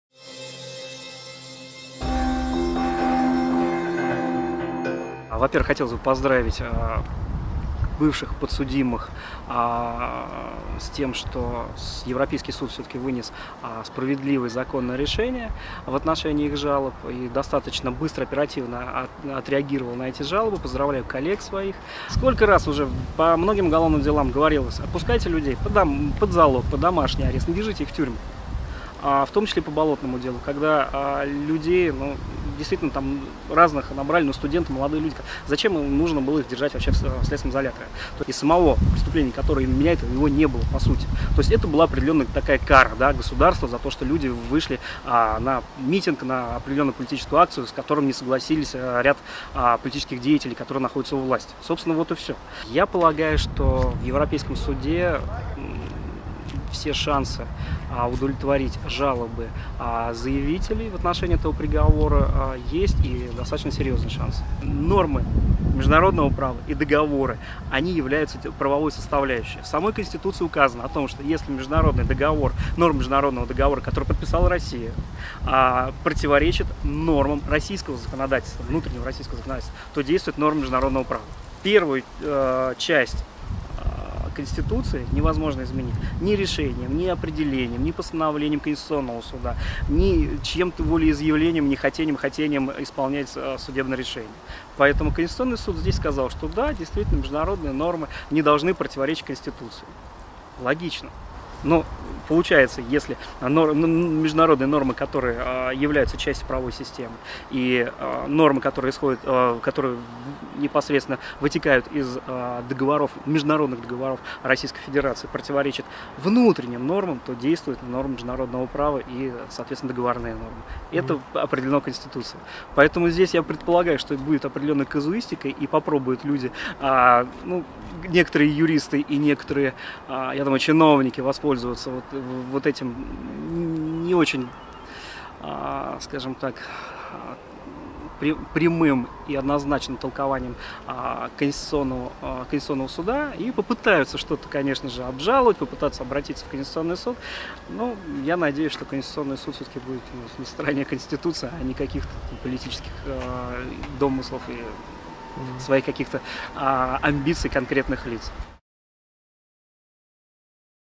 Сергей Бадамшин о первом решении ЕСПЧ по «Болотному делу»
Европейский суд по правам человека вынес первое решение по «Болотному делу». Он признал, что трое фигурантов необоснованно содержались в СИЗО в течение всего следствия. Адвокат Сергей Бадамшин прокомментировал это решение и рассказал, может ли Россия воспользоваться решением КС о допустимости игнорировать решение ЕСПЧ, если оно противоречит Конституции.